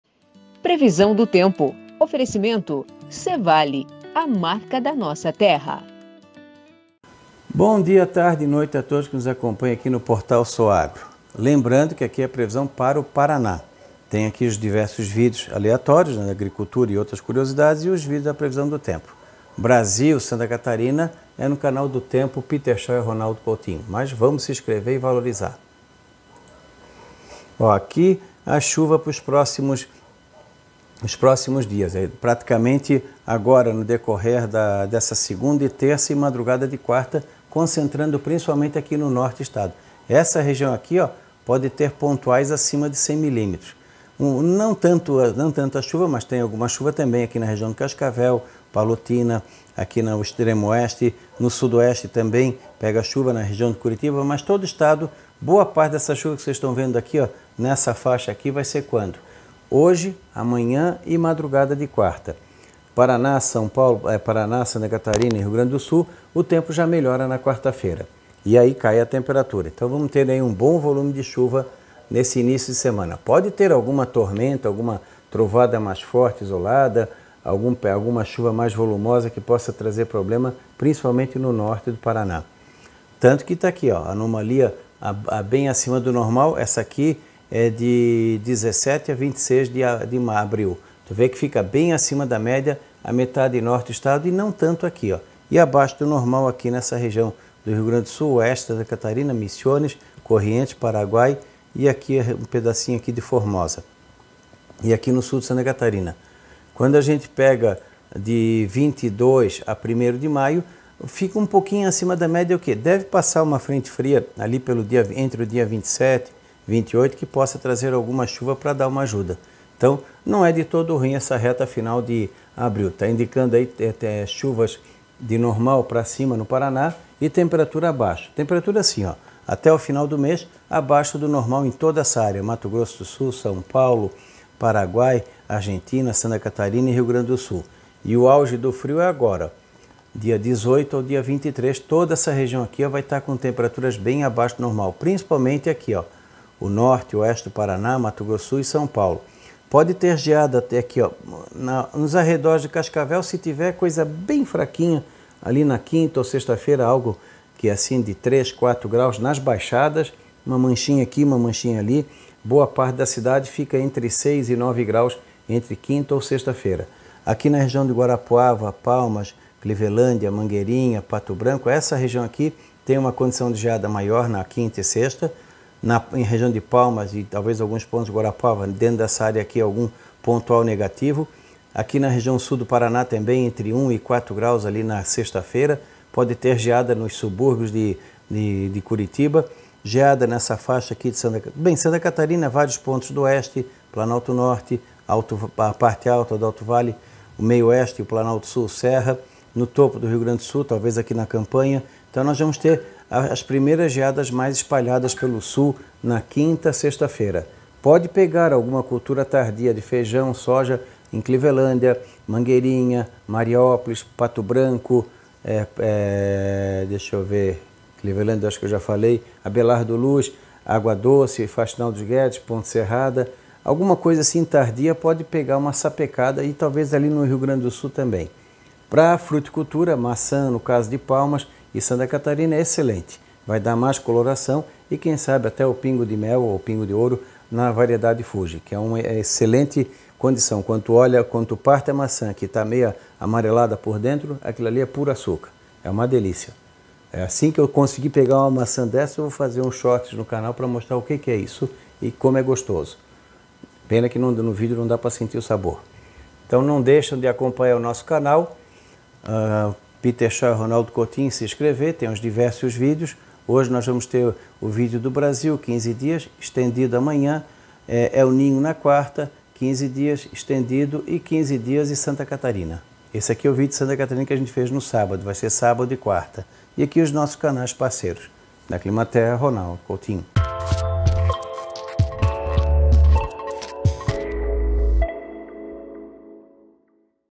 Previsão é de semana com baixas temperaturas e possibilidade de geadas no PR
"Podemos ter registro de geada na quinta ou sexta-feira em algumas regiões do Paraná. Principalmente na região de Guarapuava, Palmas, Mangueirinha, Clevelândia, Pato Branco. No Sul do Paraná, há previsão de temperaturas entre 1 e 4 graus na sexta feira." afirmou o Engenheiro Agrônomo, Ronaldo Coutinho.